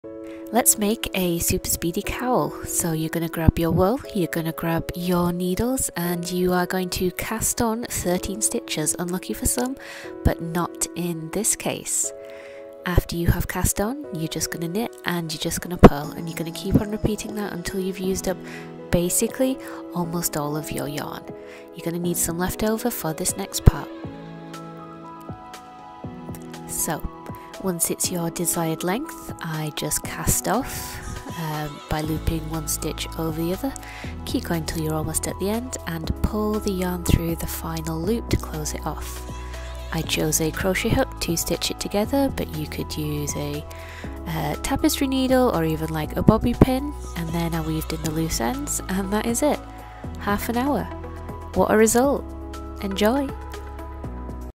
Quick and Easy Cowl DIY | last minute knitted gift
Need the perfect last minute knitted gift? This quick and easy DIY cowl will take you less than an hour, is super warm, super comfy and super cute.

I love giving a knitted gift for the Holidays, especially designs as quick and easy as this one!

If you liked the video please let me know by liking or commenting- it makes me smiley inside.

Lady Cedar